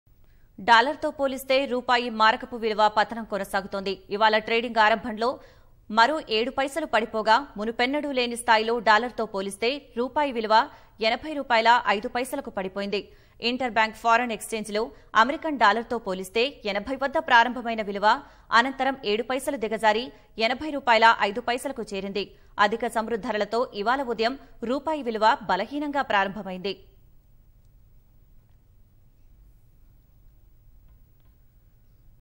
Indian Rupee Falls to 80/Dollar | రూపాయి మారకపు విలువ పతనం
డాలరుతో పోలిస్తే రూపాయి మారకపు విలువ పతనం కొనసాగుతోంది. ఇవాళ ట్రేడింగ్
ఆరంభంలో మరో ఏడు పైసలు పడిపోగా మునుపెన్నడూ లేని స్థాయిలో డాలరు తో పోలిస్తే రూపాయివిలువ 80 రూపాయల ఐదుపైసలకు. పడిపోయింది. ఇంటర్ బ్యాంక్
ఫారిన్ ఎక్ఛేంజ్ లో అమెరికన్ డాలర్ తో పోలిస్తే 80 వద్ద ప్రారంభమైన విలువ... అనంతరం ఏడు పైసలు దిగజారి 80 రూపాయల ఐదు పైసలకు. చేరింది. అధిక చమురు ధరలతో
ఇవాళ ఉదయం రూపాయి విలువ బలహీనంగా ప్రారంభమైంది.